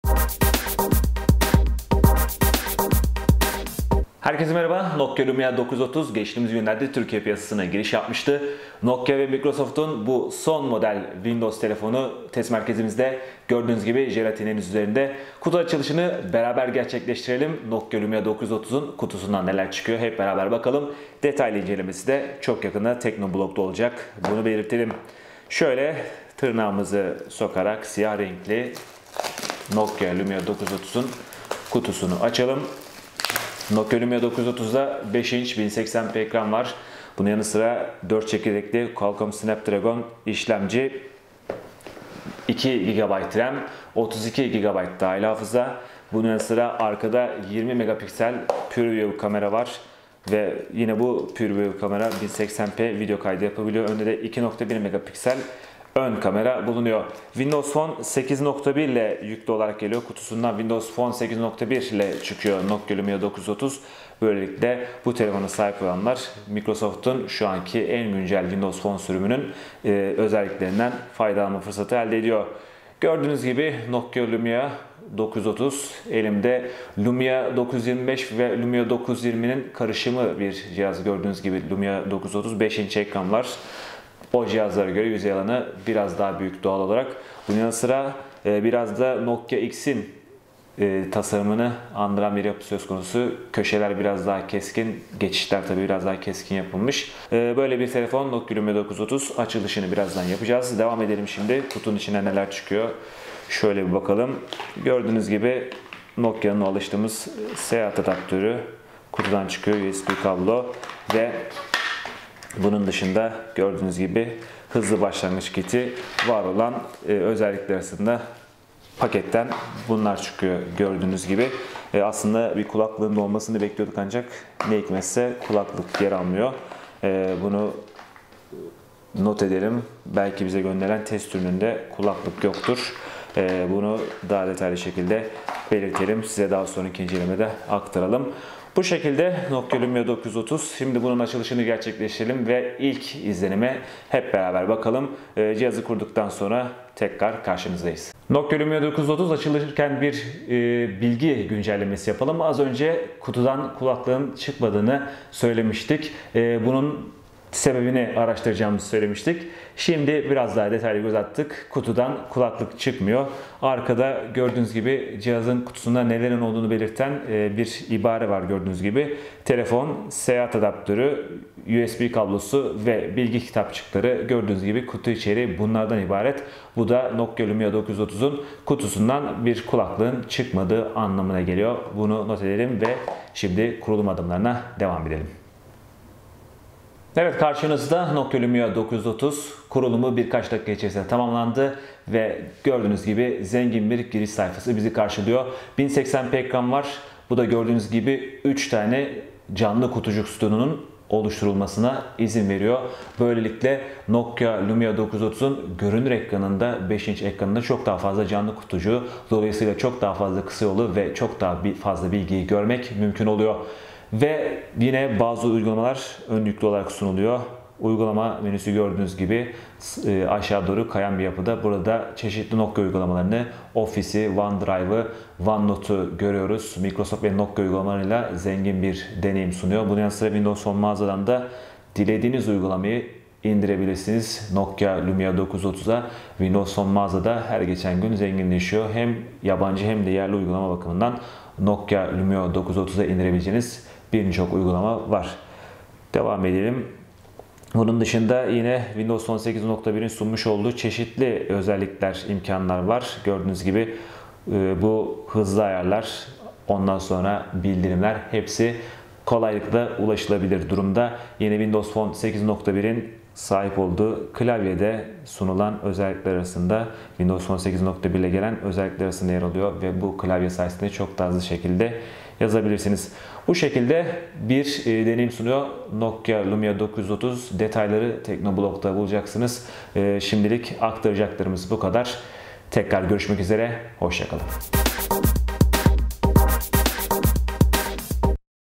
Nokia Lumia 930 Kutusundan Çıkıyor
Microsoft ve Nokia'nın yeni Windows telefonu Nokia Lumia 930 Türkiye'de satışa sunuldu. Test merkezimize ulaşan bu akıllı telefonun kutusunu birlikte açıyoruz. Detaylı incelemesi de çok yakında Teknoblog'da olacak.